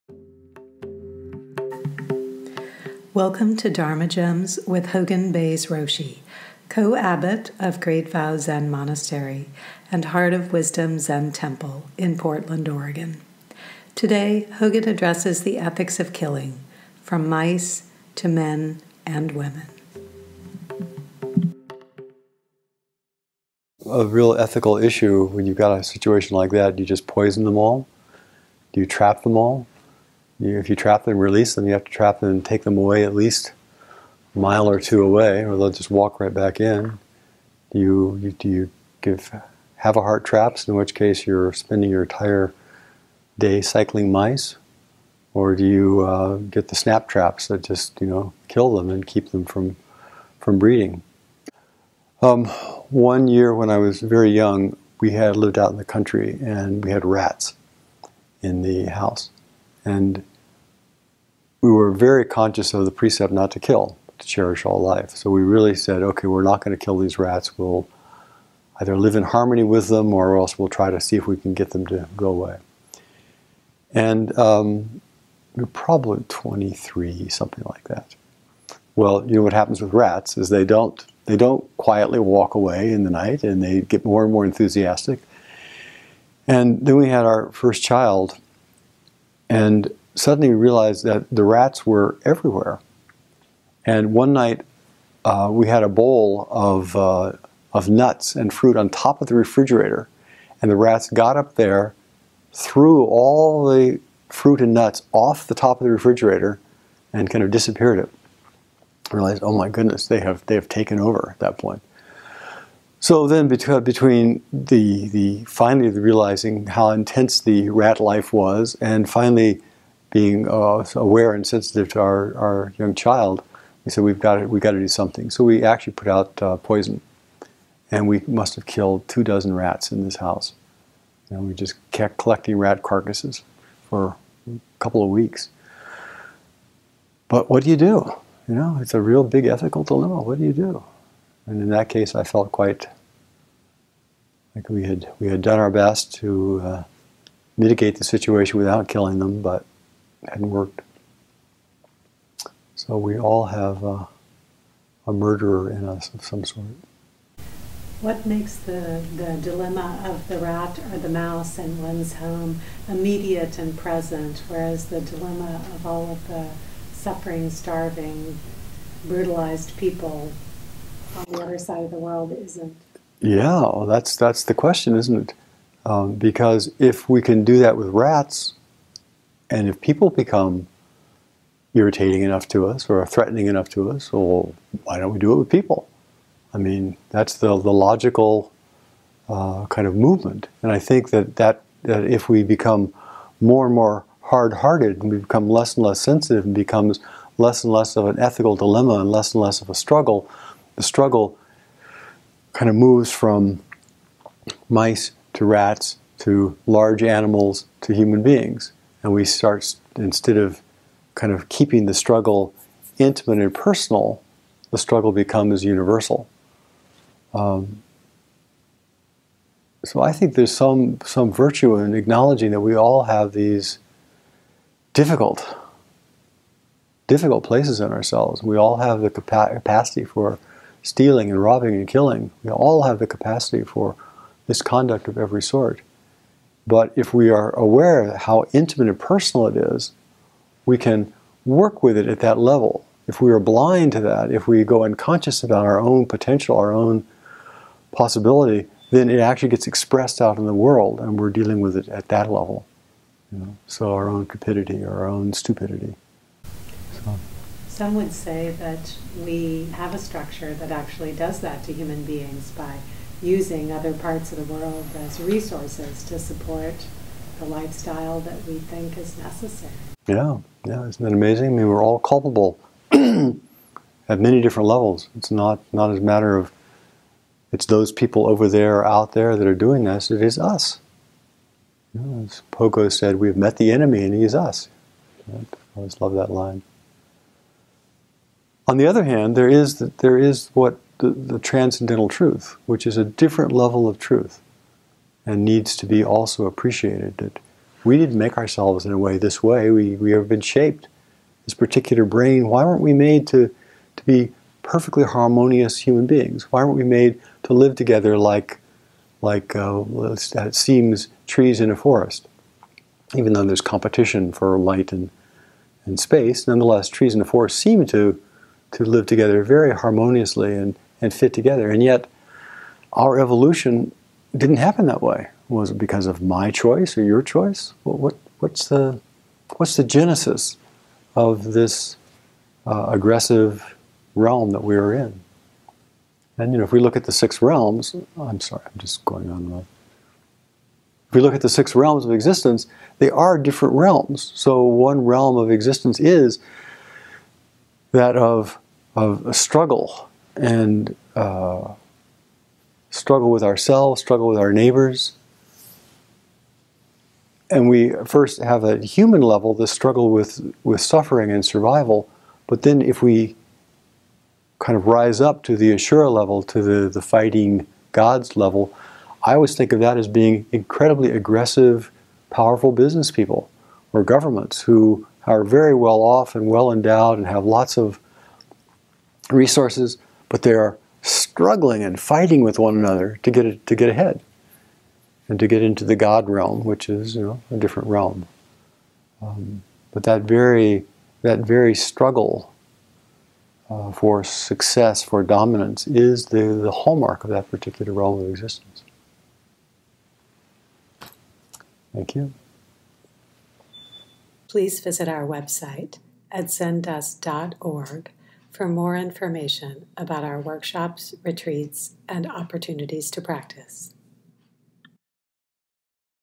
The Precept Against Killing From Mice to Men and Women- Hogen Bays Roshi
Hogen addresses the first precept of the bodhisattva--the vow not to kill but to cherish all life.  He reminds us how easy it is once we harden our hearts to other creatures to lose compassion for our fellow human beings.  What do we do when the mice invade the house?